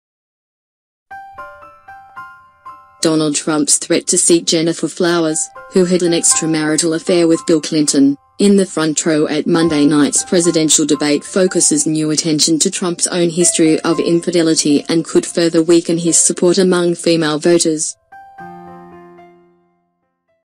Donald Trump's 'invite' to Gennifer Flowers highlights his own history of infidelity
Donald Trump's threat to seat Gennifer Flowers, who had an extramarital affair with Bill Clinton, in the front row at Monday night's presidential debate focuses new attention to Trump's own history of infidelity and could further weaken his support among female voters.

Trump's suggestion, delivered Saturday via a Tweet, was widely seen as an effort to rattle Clinton ahead of Monday's high-stakes debate and fired up some of his hardcore supporters. Trump's campaign reversed course Sunday, saying Flowers was not going to be invited to the debate.

Even so, some analysts questioned whether it was wise for Trump to even bring up the issue in an attempt to smear Hillary Clinton for the failings of her husband, especially given his own playboy past.

"Why is he doing this to Hillary Clinton?" said Mary Anne Marsh, a Democratic strategist. "This is not about her behaviour, it's something that happened to her. Women will not see this as fair.

"His base will love it, but the very voters he needs - white, college-educated women - will not," she said. "It will hurt him."

Polls show Trump and Clinton virtually deadlocked among all voters, but Clinton has far greater support among women. Among likely voters who are women, Clinton leads by 19 points, 55 per cent to 36 per cent, according to a Washington Post-ABC News poll released Sunday. It found Clinton up 2 points overall among likely voters.

Trump is currently losing among likely white women voters with a college degree by 25 points, according to the same poll. In 2012, Republican nominee Mitt Romney won that group by six points. Trump's deficit among white, college-educated women is unprecedented in exit polls going back to 1980.

Katie Packer, a Republican strategist who worked on Romney's 2012 presidential campaign and who ran an anti-Trump PAC during the GOP primaries, said research shows that even "women who aren't immediately inclined toward Hillary" are sympathetic toward her "when people punish her for her husband's infidelity."

"They feel it's very sexist to hold her responsible for his bad behaviour," she said. About Trump, she said, "He is a serial philanderer, too, and has publicly bragged about it. He has no moral high ground."

Trump - who has been married three times - separated from his first wife, Ivana, after his affair with Marla Maples became widely known. Trump and Ivana finalised their divorce in 1992. Trump married Maples in December 1993, two months after she gave birth to a daughter. Trump divorced Maples in 1999 and married Melania Knauss in 2005.

After his second divorce and at other times when he was single, Trump was frequently seen out on the town with different women. He appeared on the cover of Playboy magazine, wrote in his books about all the women chasing him and publicly boasted about his sex life.

"I haven't thought about it," Trump said in April when asked if he thought his affair with Maples while he was married to his first wife would come up in the general election if Bill Clinton's affairs were aired again.

"I don't see it as a very big situation. It was a long time ago," he said. "I may be wrong. I am learning everything I do is a big deal."

Trump insists his affair with Maples, who was quoted in tabloids saying "Best Sex I've Ever Had!" about Trump, is not like Bill Clinton's infidelities. Those, Trump said in the interview, are "from another planet."

Trump insisted that even though his marriage with Ivana had not officially ended, "it was over" when he began seeing Maples.

But his first wife was stung. Ivana Trump - who has kept an exceptionally low profile during the 2016 campaign - said at the time of the affair that she overheard her husband talking to Maples on the telephone when the couple were staying in an Aspen hotel with their three children at Christmas time in 1989. Ivana and Maples then famously had a public spat over Trump on the ski slopes.

Mike Murphy, a Republican strategist who backed Jeb Bush in the primaries, said Trump would have been wiser to tweet that he was bringing a woman widowed by the terrorist attack in Benghazi, Libya, to the debate. While Trump made the kind of "media noise" that he loves to create, Murphy said, this move was "typical Trump: all impulse and no strategy."